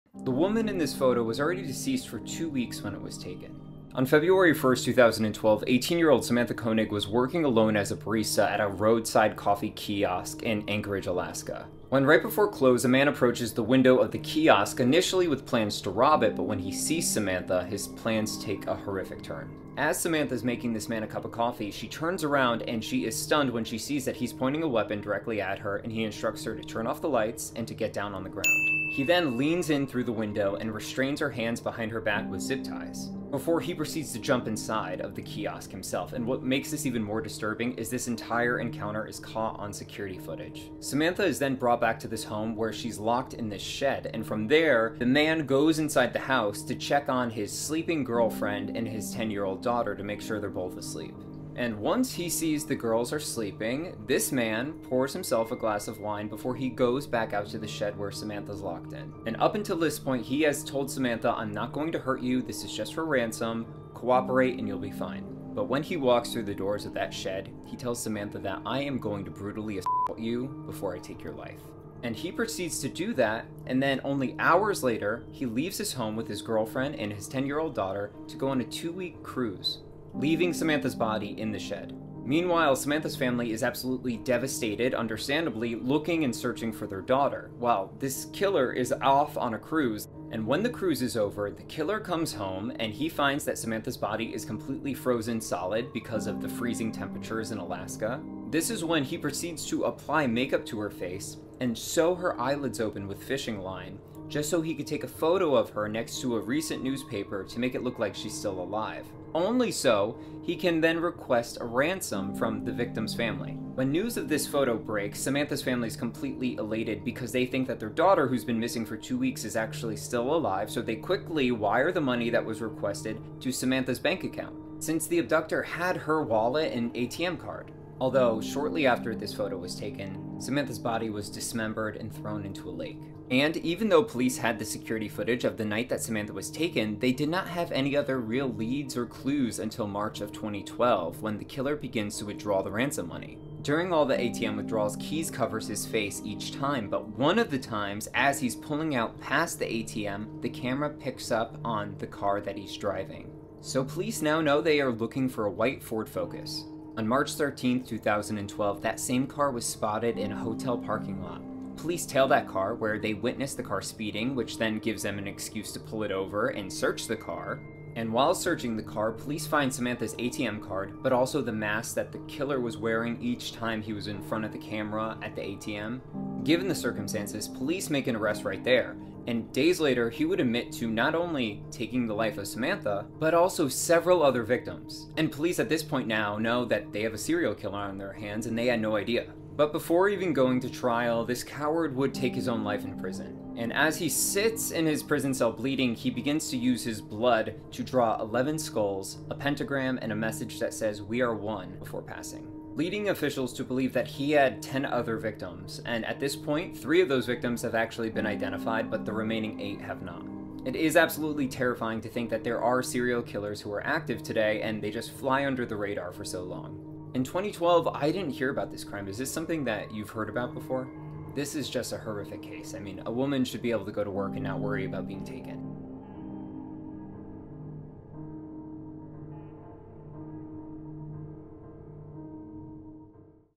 Killer Sewed Victims Eyelids Open To Make Her Look “Alive”😳
The heartbreaking and horrific truecrime story of Samantha Koenig

On February 1st, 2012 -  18 year old Samantha Koenig was working alone as a barista at a roadside coffee kiosk in anchorage Alaska. When right before close a man approached the window with plans to rob it, but when he set eyes on Samantha his plans took a horrific turn.

As Samantha was making the man a coffee he pulled a weapon out and proceeded to restrain her, before taking her to a shed behind the home that his girlfriend and daughter were sleeping in. From this point this story gets extremely dark.